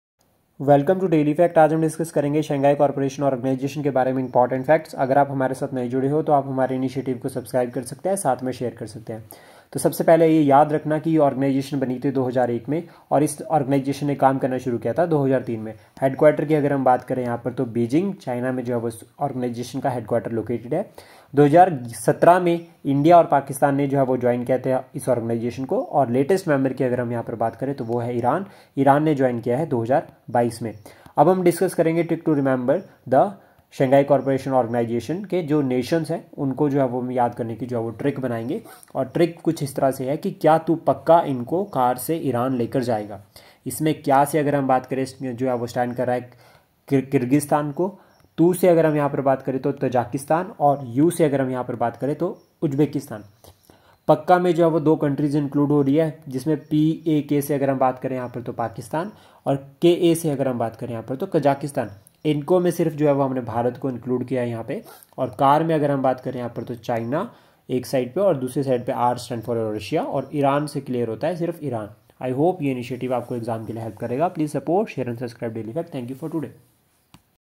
Trick to remember the Shanghai cooperation organisation members II Daily Facts
The SCO has eight members, including China, India, Kazakhstan, Kyrgyzstan, Pakistan, Russia, Uzbekistan, and Tajikistan. Iran is set to join the SCO.
Afghanistan, Belarus, and Mongolia are observers who would like to follow Tehran.